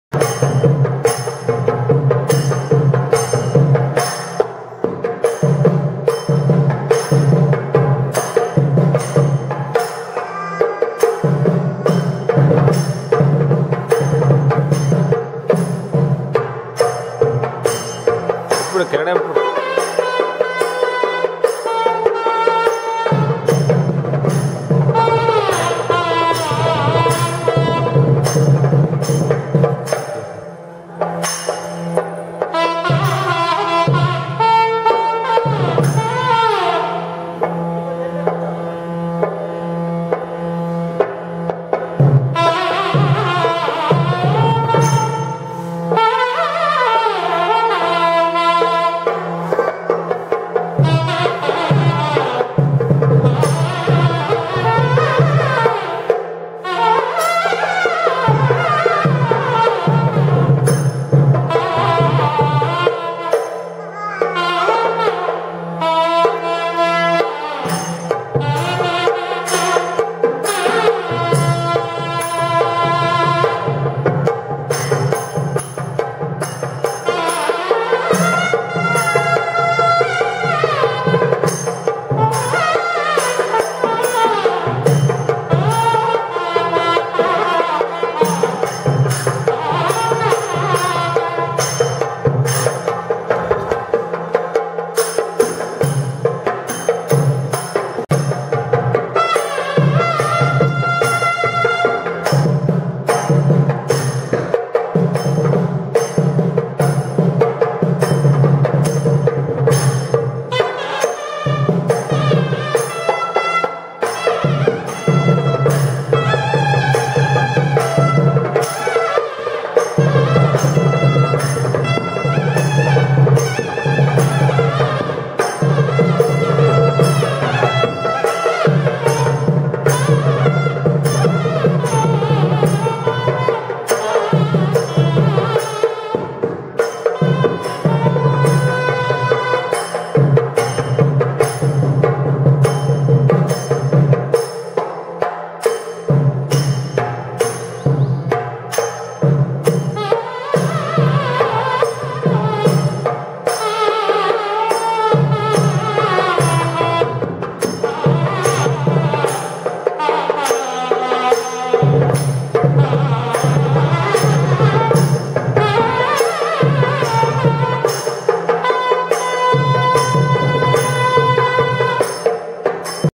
Nesara -Music instincts
As dusk approaches, the Nadaswara music group performs at the Jain temple located in a small hillock at Dharmsthala.
Nesara (eleven months then) displays a passion for musical instruments. As always she listens with devotion, involvement and enthusiasm. In this serene though humid atmosphere, the cool breeze permeates through the temple’s stone pillars. And then the music and Nesara’s keeping to the beat soothes our body and minds.

- Sunita / Raghu